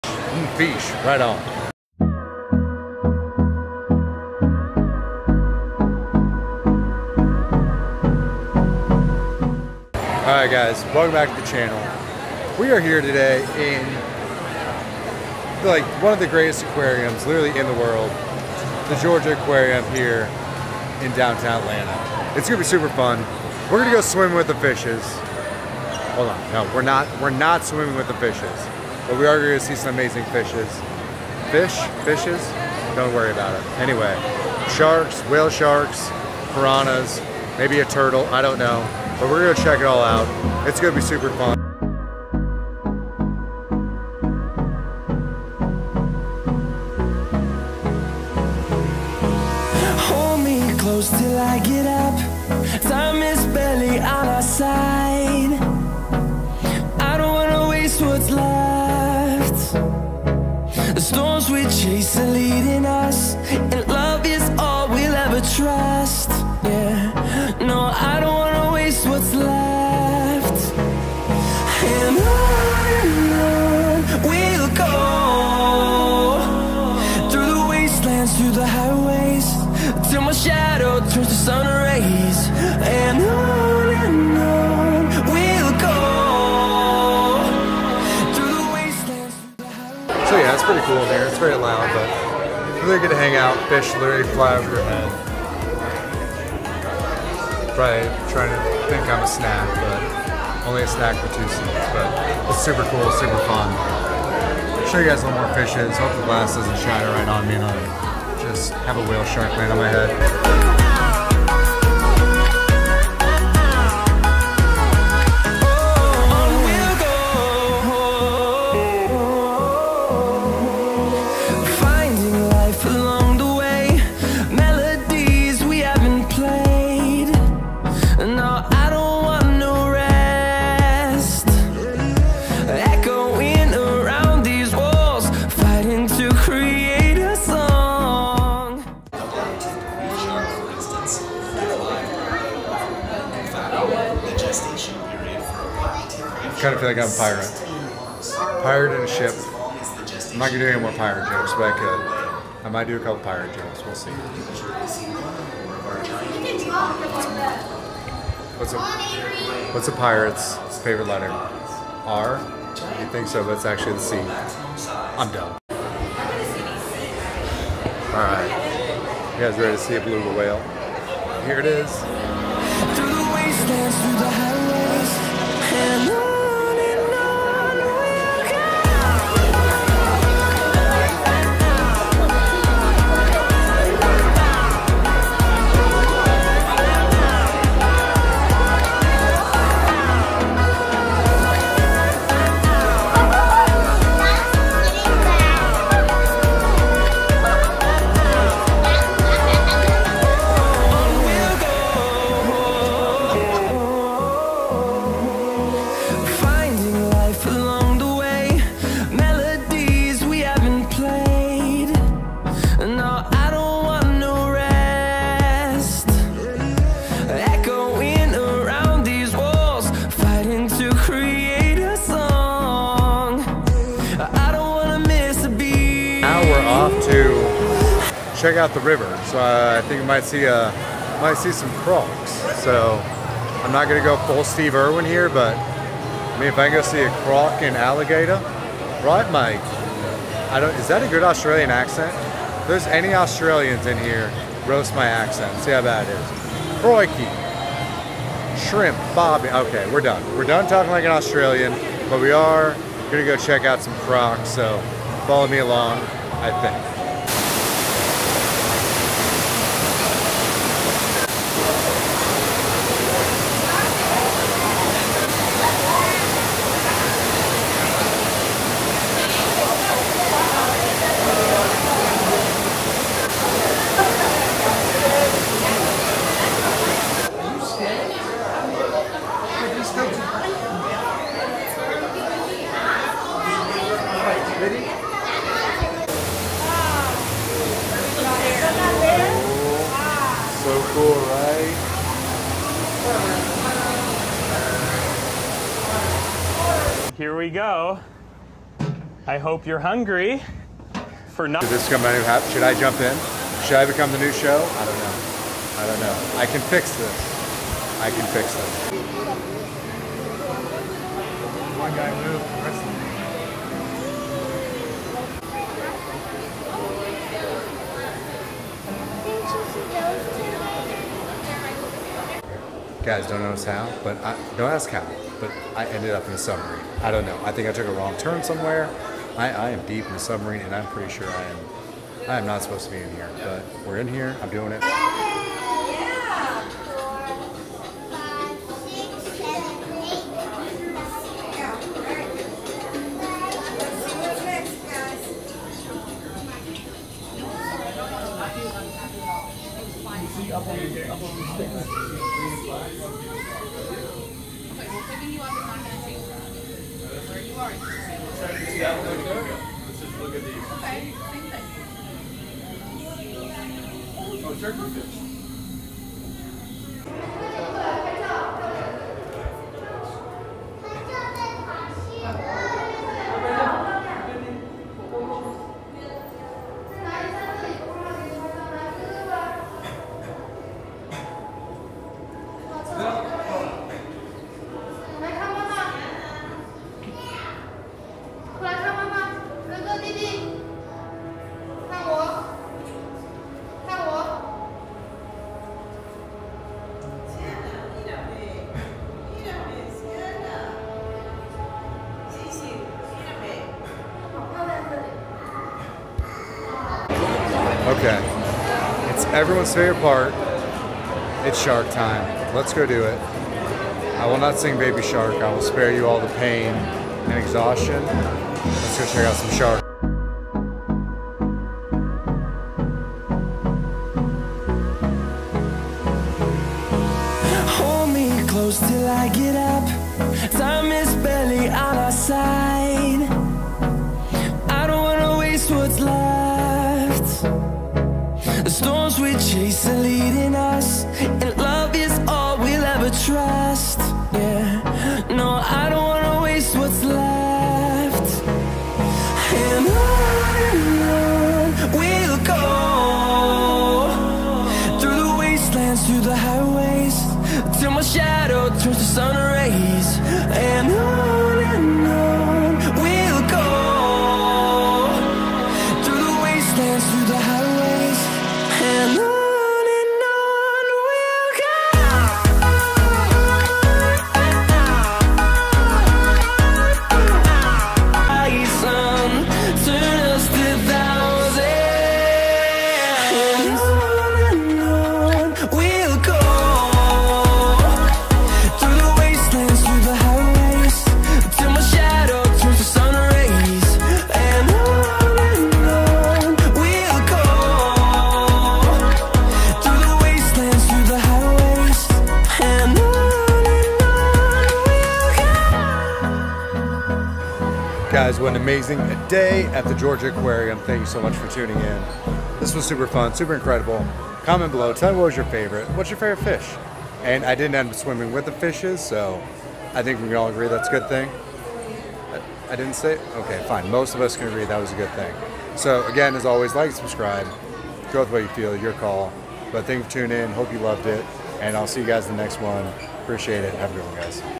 Georgia Aquarium: Mmm Fish Right On
Took a trip to the Georgia Aquarium in Atlanta, didn't go swimming with the fishes.